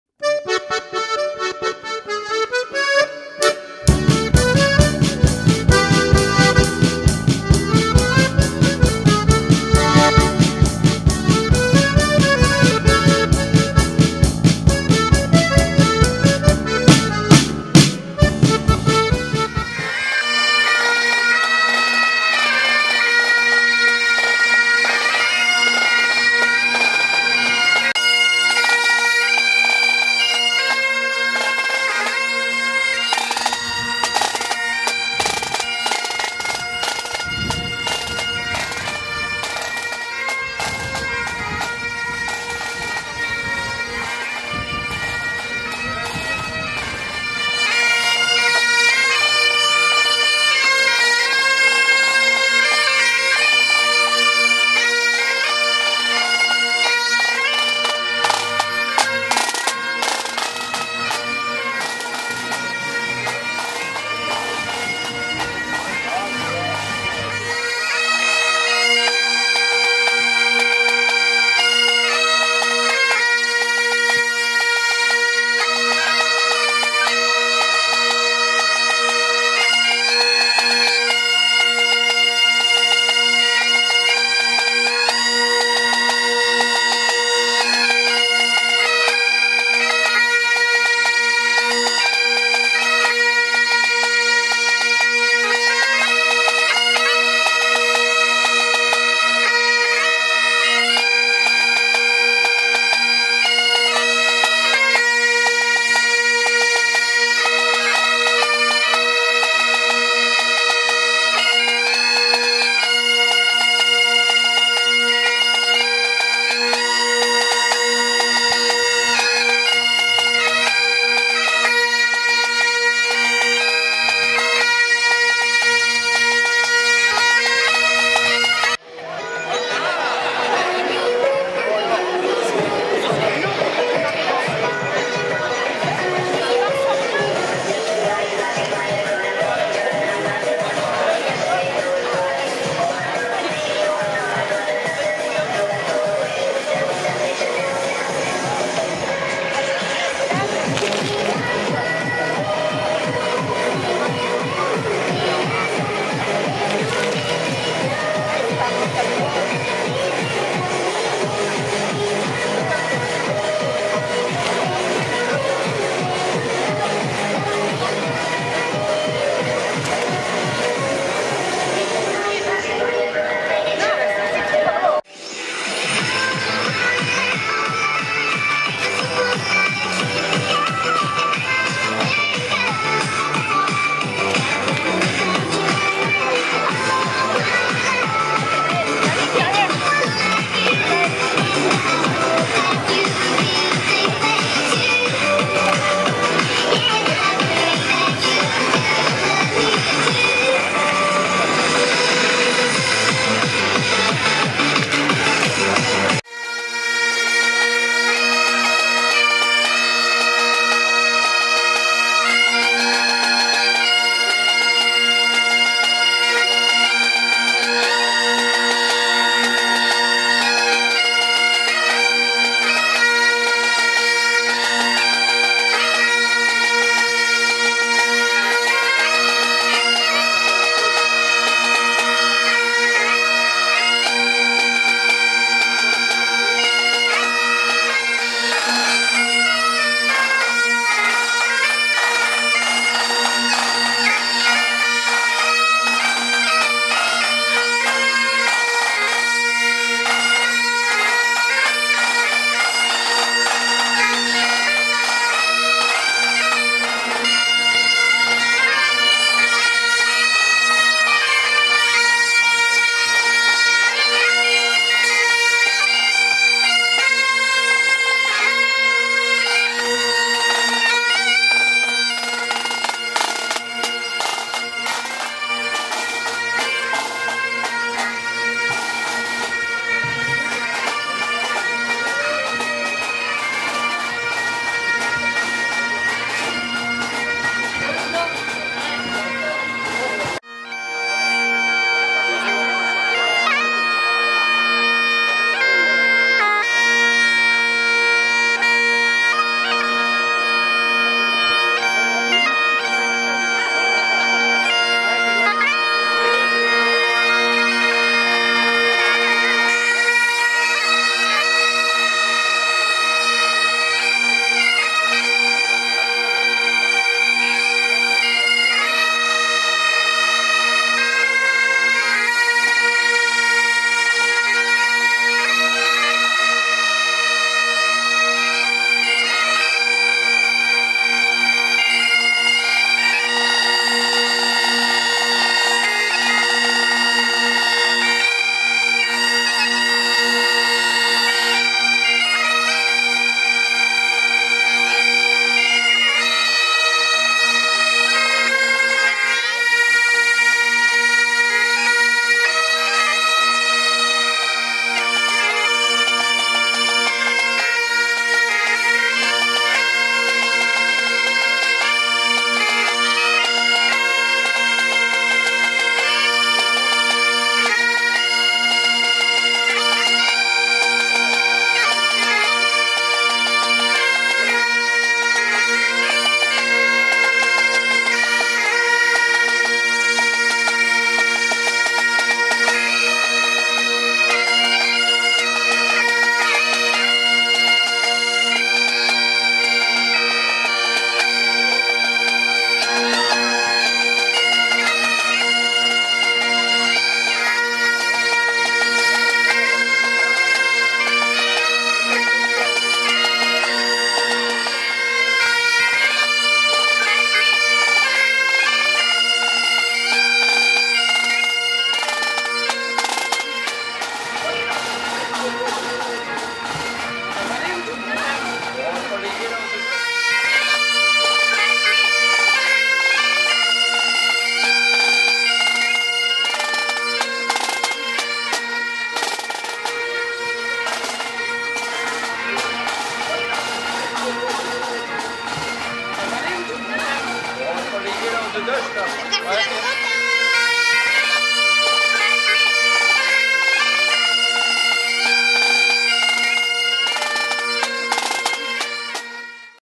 T'Chout Jaques, Fête de Voyennes (Somme 80) le 28 aout 2011
Partcipation des géants T'Chout Jaques, Armandine et dudule à la fête de Voyennes (Somme 80) le 28 août 2011. Avec la participation des Cornemuses d'Hombleux et les Majorettes "Capucines" de Vecquemont.